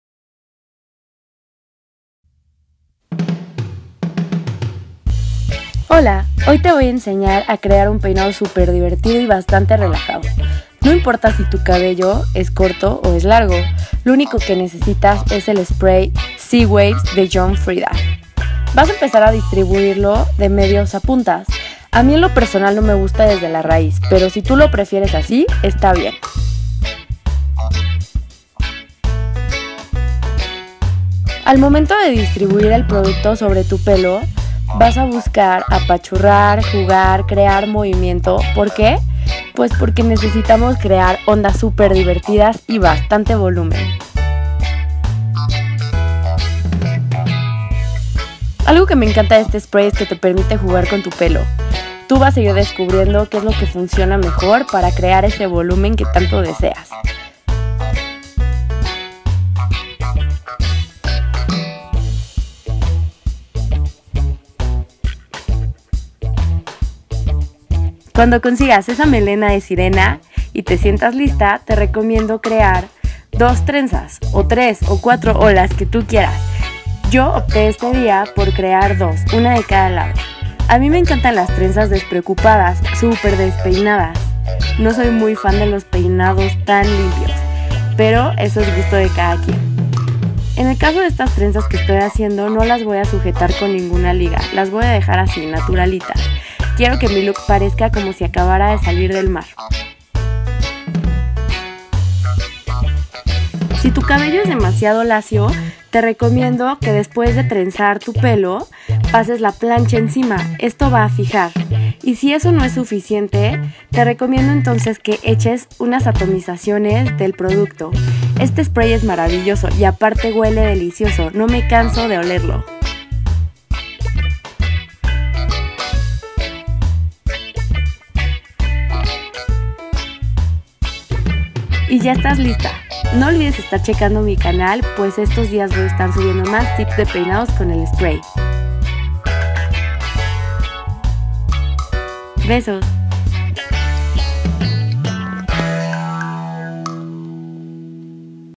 Péinate como una BeachBabe!
Hola! Hoy les traigo un mini tutorial para crear un peinado súper sencillito pero top! Ya casi llega el verano y no hay nada mas hot que una BeachBabe!.

John Frieda te ayuda a crear esas ondas naturales de surfer, te apuntas?.

En estos días estaré subiendo más tutoriales para que te vuelvas una experta en BeachWaves!

No olvides checar la reseña completa de estos productos en mi blog